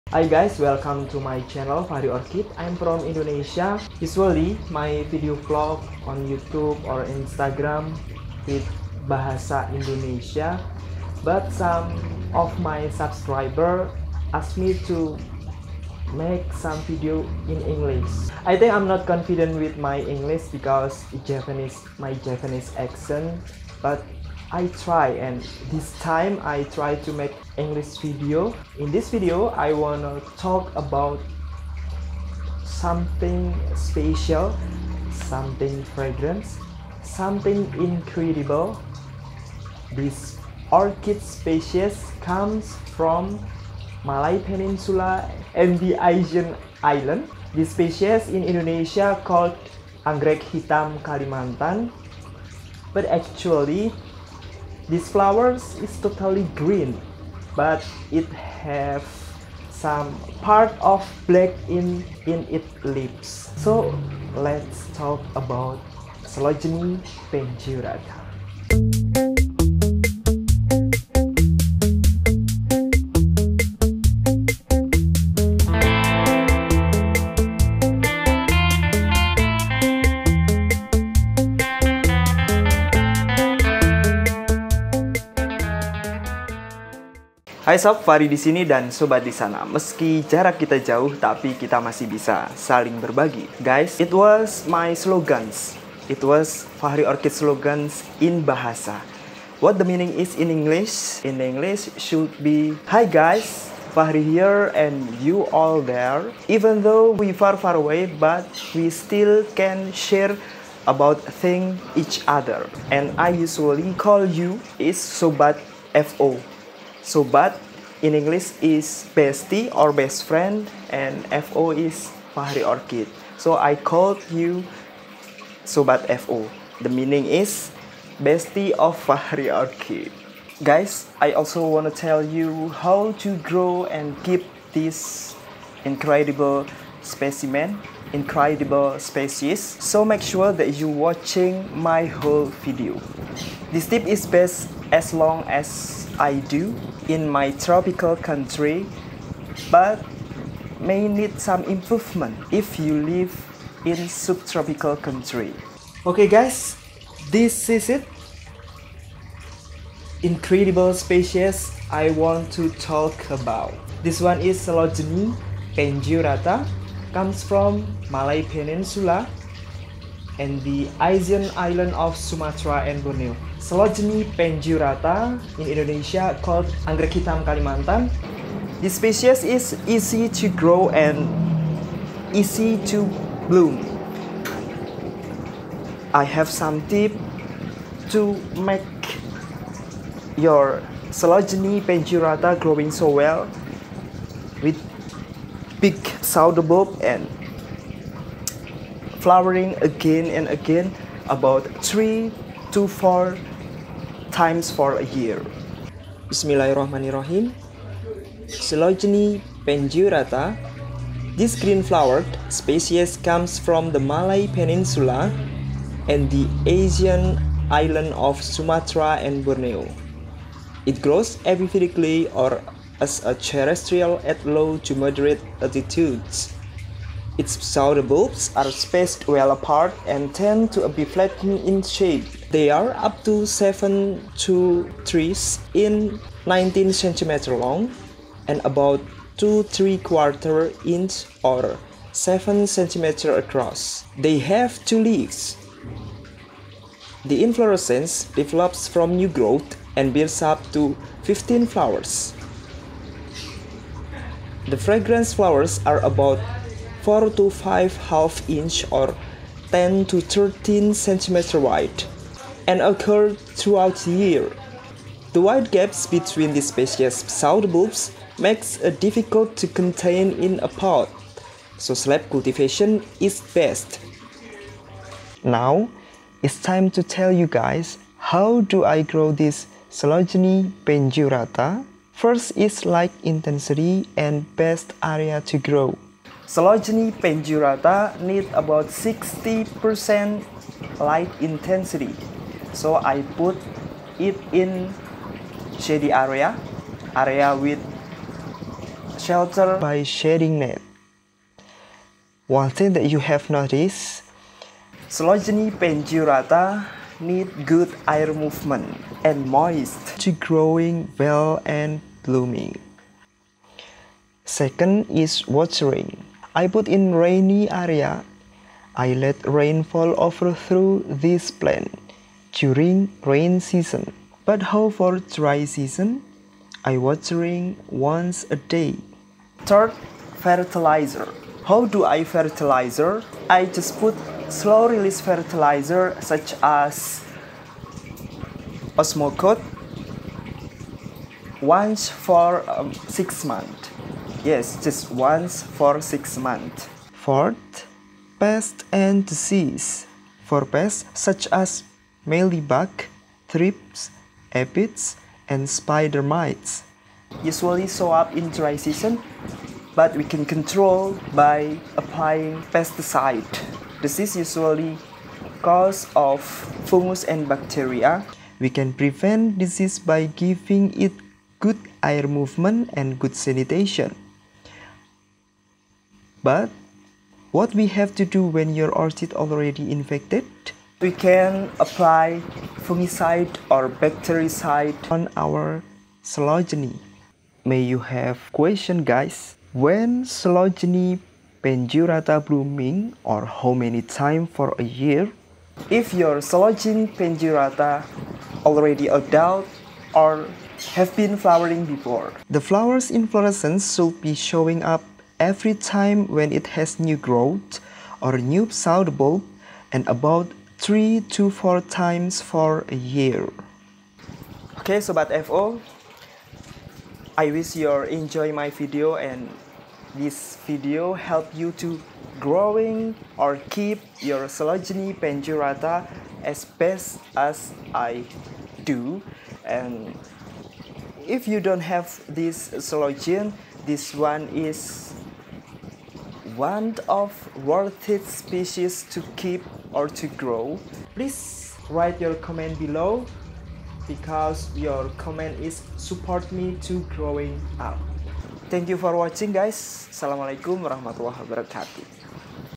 Orchid care Tips to blooming Coelogyne pandurata, Black orchid for beginners
Coelogyne pandurata is incredible Orchid species with beautiful huge flower and fragrance, in this video I share to you some tips to grow and talk about anything of this Orchid.